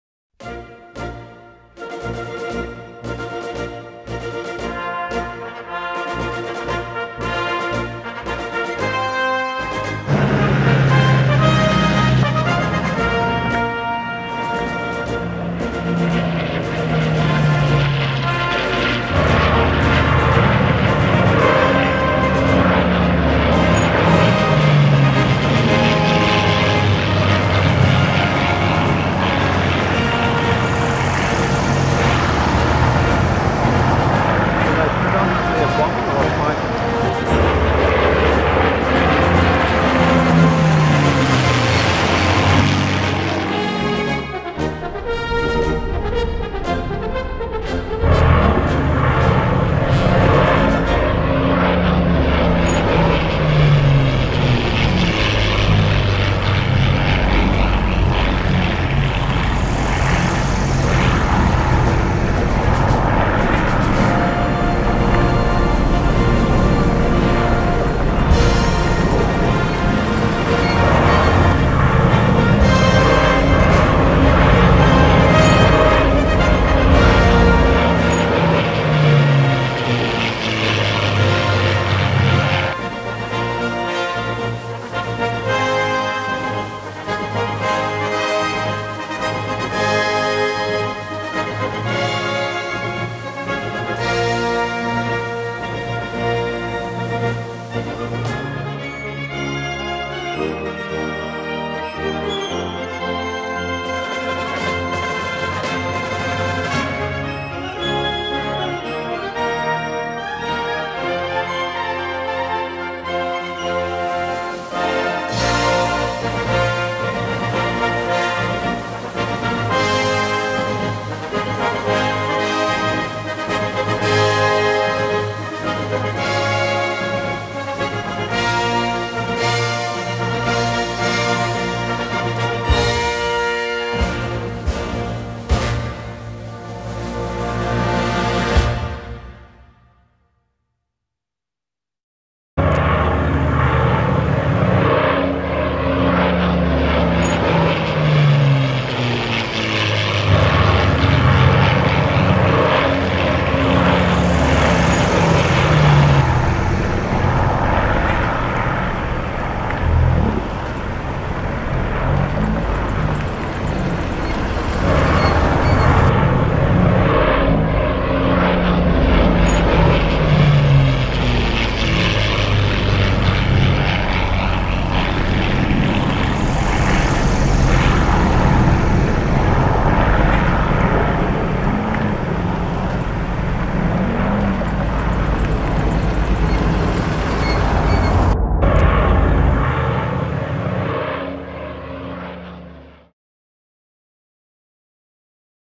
mosquito
In world war 2 our father flew a fighter bomber called a MOSQUITO, he was 20 years old. The missions over hostile lands were flown mainly at night at very low altitudes, he said it was not unknown for some planes to return with branches caught up in their tail wheel . He was shot down but evaded capture thanks to the bravery of the French who sheltered him till he got back to active service.